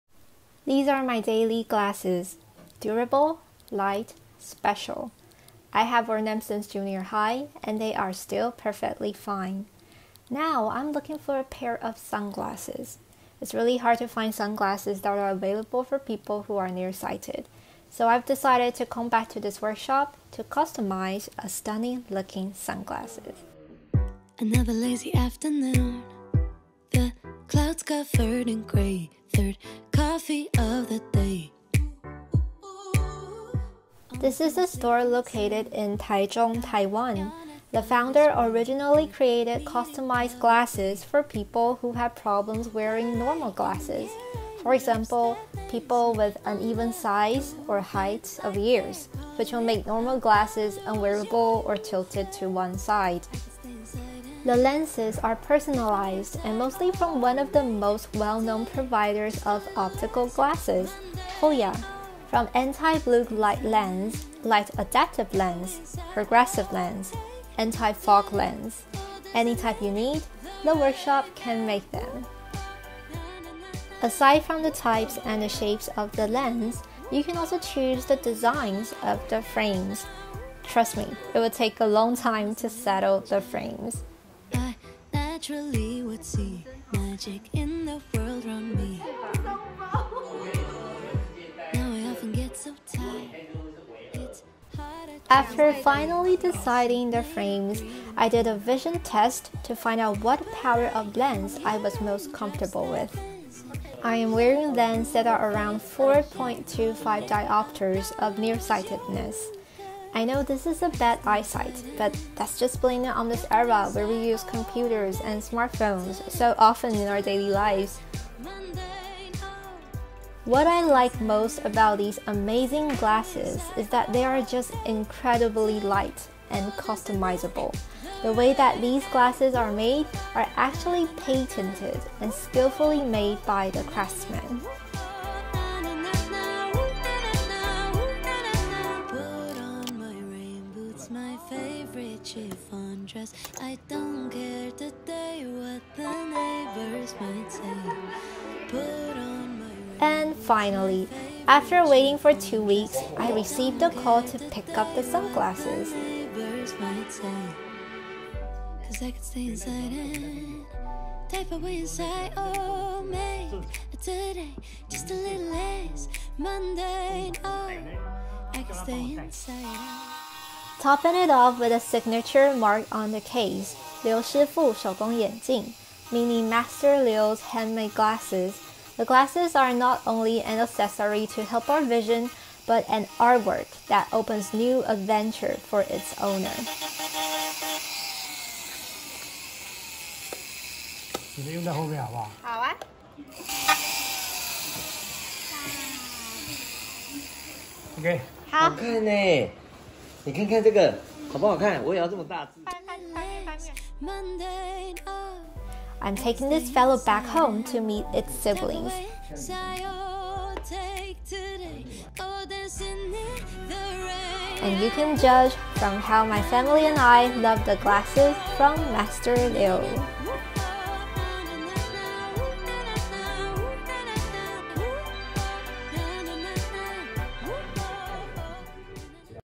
[HappyLife] Specular Handmade Glasses Workshop ft. 老劉師傅手工眼鏡
One of the most well-known spectacle workshop that you must know! Highly recommended to people who ever plans to customize their eyeglass.
p.s. It will be so hard to look for glasses from other brands once you tried it at this workshop. A real example right here!

老劉師傅手工眼鏡
434台中市龍井區藝術南街25號 (東海大學藝術街)
No. 25, Yishu S St, Longjing District, Taichung City, 434